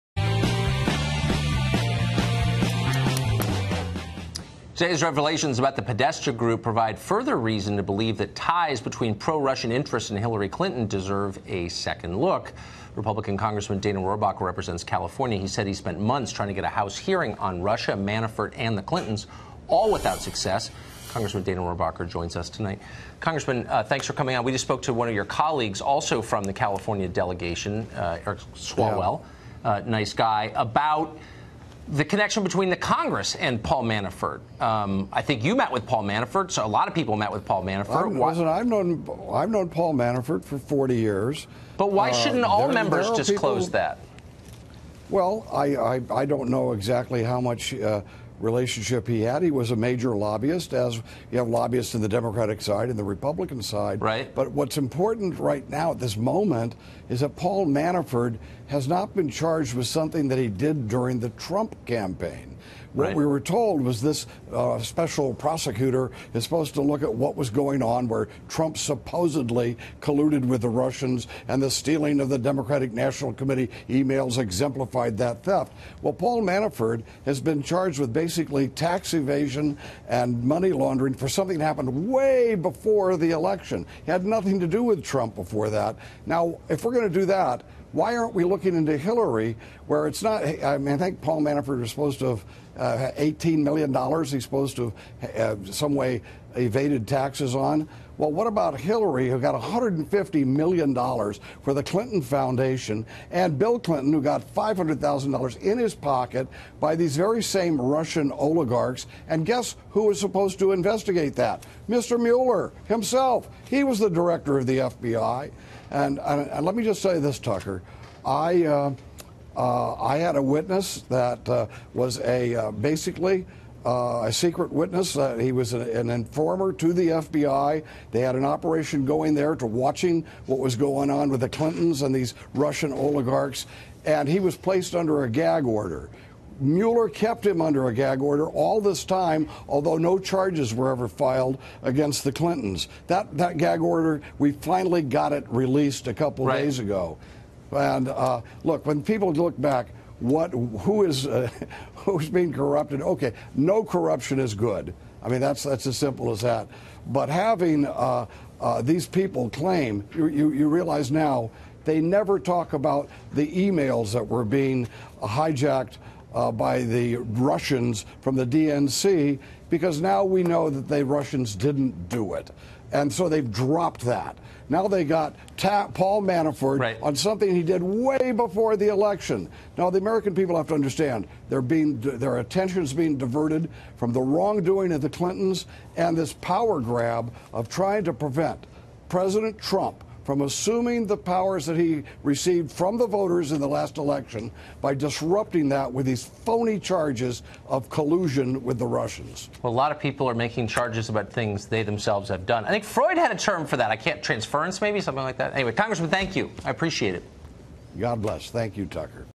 [ Tucker Porn ] Congressman Hearing on Clintons Russia long overdue
Congressman Hearing on Clintons Russia long overdue